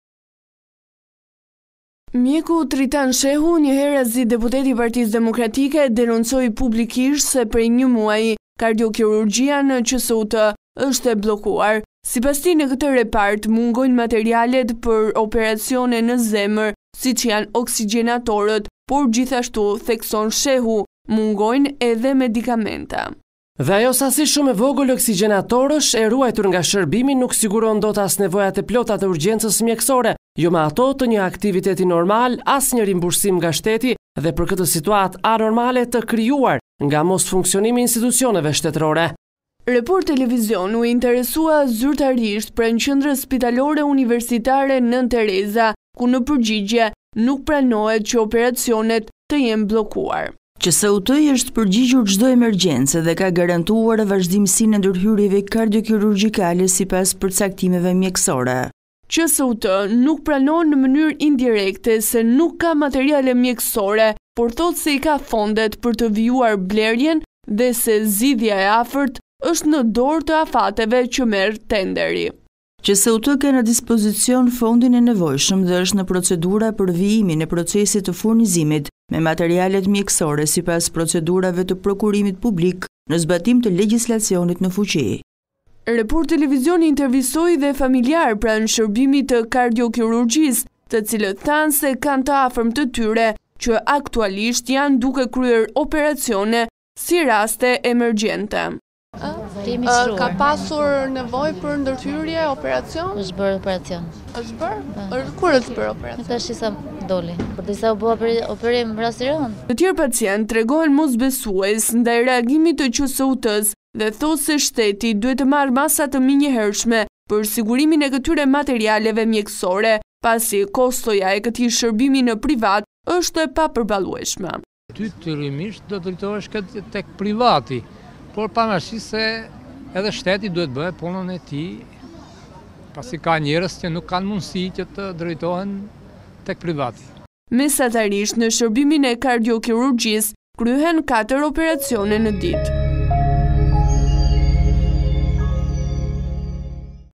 Denoncimi i Shehut për bllokimin e kardiologjisë, reagon QSUT: I jemi përgjigjur çdo emergjence!
reportTV lajmeshqip lajme Report Tv u interesua pranë QSUT-së duke kërkuar një reagim pas denoncimit të mjekut dhe deputetit Tritan Shehu se si pasojë e mungesës së materialeve mjekësore, janë bllokuar prej 1 muaji operacionet e zemrës. QSUT thotë se urgjencat dhe ndërhyrjet në zemër janë garantuar dhe shton se në vijim pritet të mbyllen edhe procedurat për blerjen e materialeve të reja.